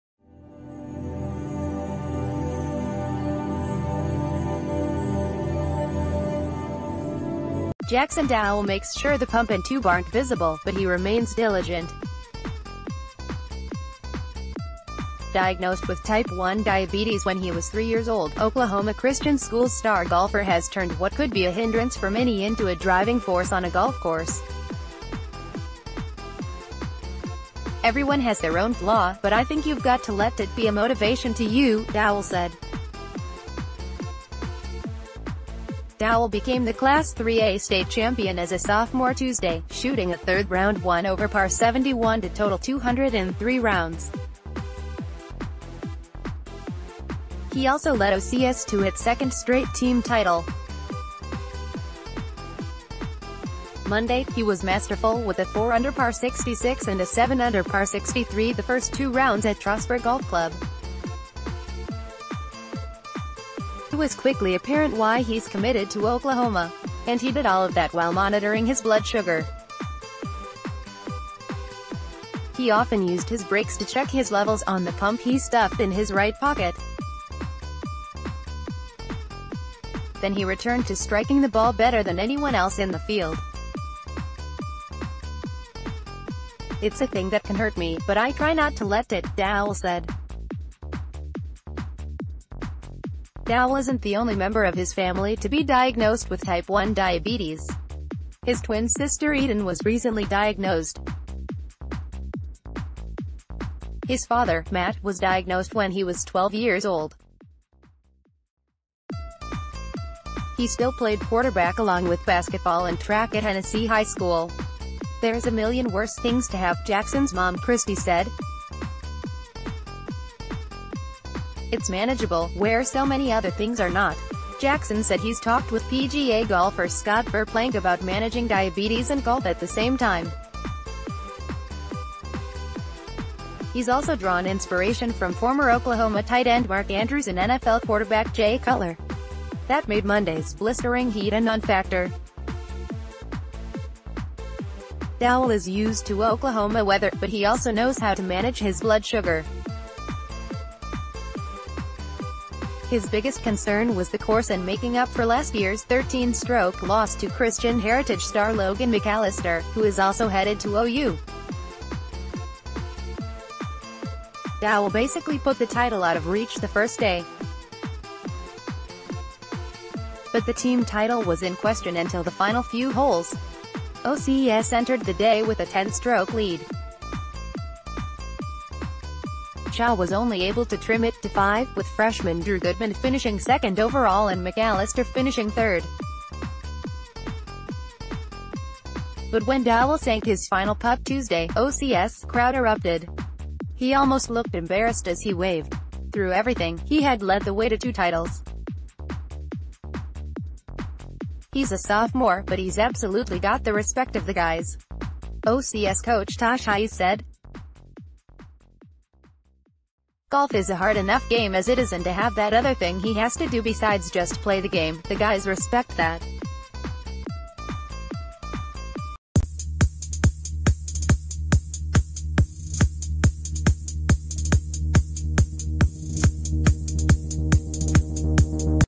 Breaking News | High school golf: OCS star Jaxon Dowell doesn't let diabetes get in way of title run
Jaxon Dowell makes sure the pump and tube aren't visible, but he remains diligent. Diagnosed with Type 1 diabetes when he was 3 years old, Oklahoma Christian School's star golfer has turned what could be a hindrance for many into a driving force on a golf course. “Everyone has their own flaw, but I think you've got to let it be a motivation to you,” Dowell said. Dowell became the Class 3A state champion as a sophomore Tuesday, shooting a third-round 1-over-par 71 to total 200 in three rounds. He...